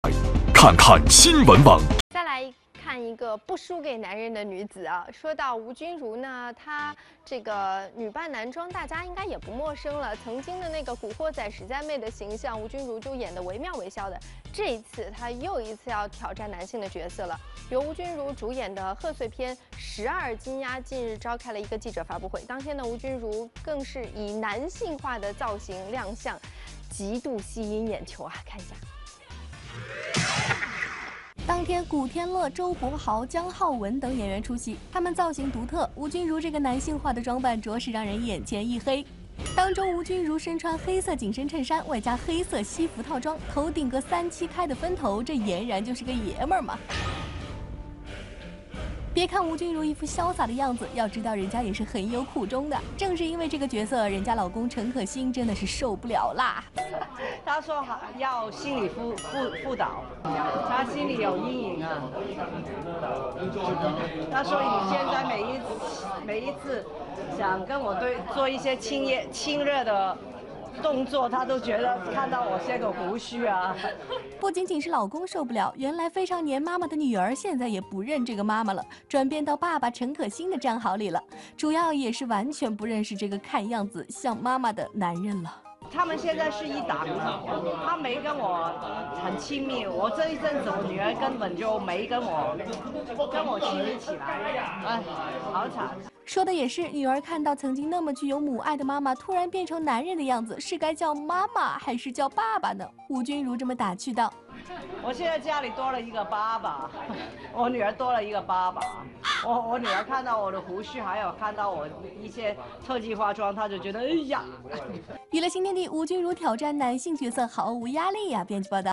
吴君如Sandra Ng扮演肌肉男 老公吃不消！
贺岁片《12金鸭》今日开放给媒体探班，饰演爆肌型男的吴君如，虽没像预告片中一样露出胸肌、腹肌及人鱼线，但仍化身男人、脸带胡须，男人味十足。